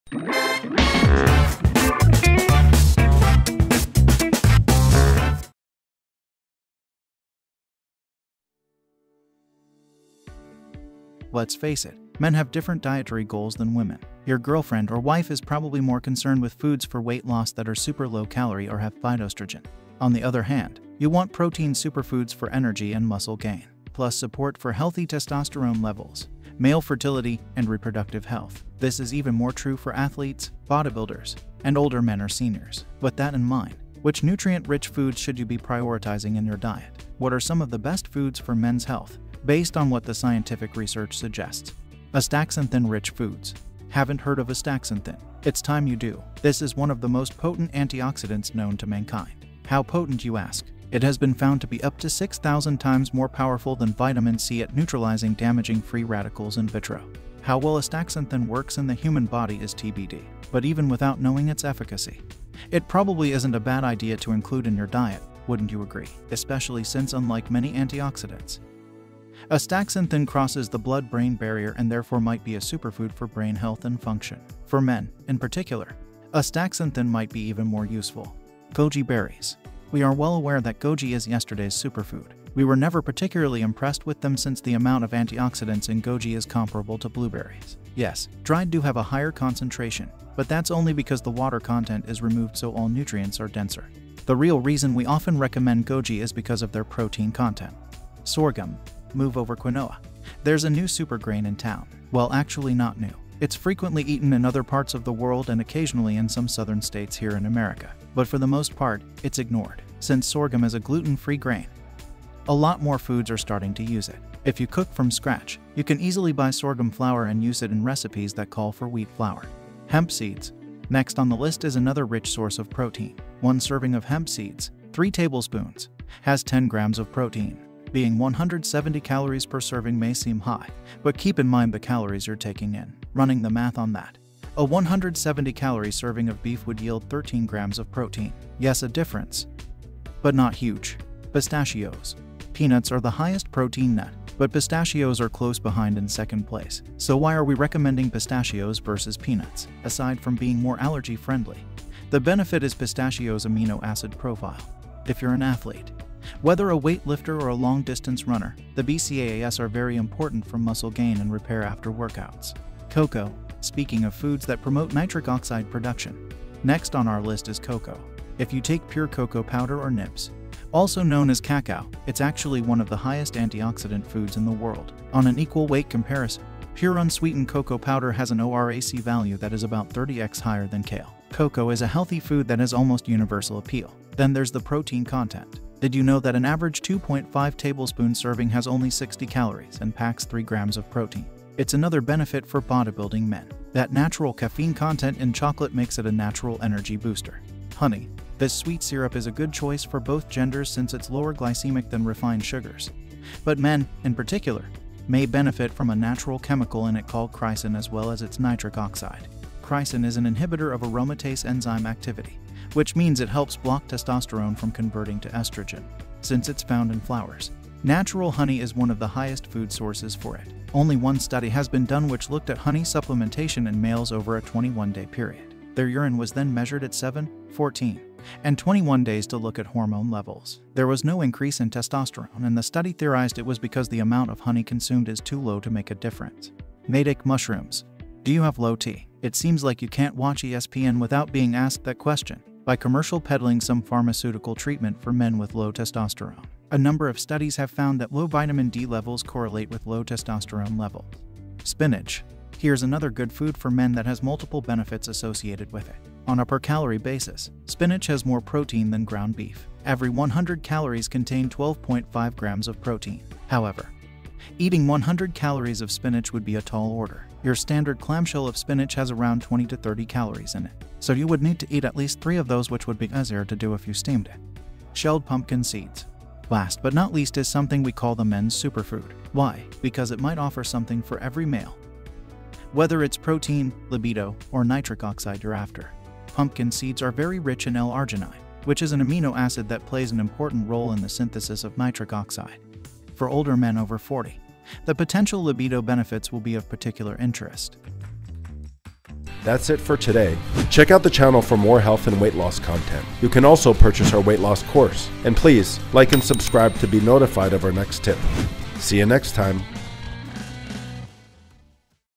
10 Superfoods For Men and 1 You Should Definitely Avoid
Let's face it: men's nutritional objectives differ from those of women. Your girlfriend/wife is probably more interested in weight reduction meals that are low in calories or contain phytoestrogens.

Protein superfoods, on the other hand, are essential for energy and muscle gain. In addition, it promotes healthy testosterone levels, male fertility, and reproductive health. This is especially true for athletes, bodybuilders, and senior citizens.

Keeping this in mind, which nutrient-dense foods should you prioritize in your diet? What are some of the finest meals for men's health, according to scientific evidence?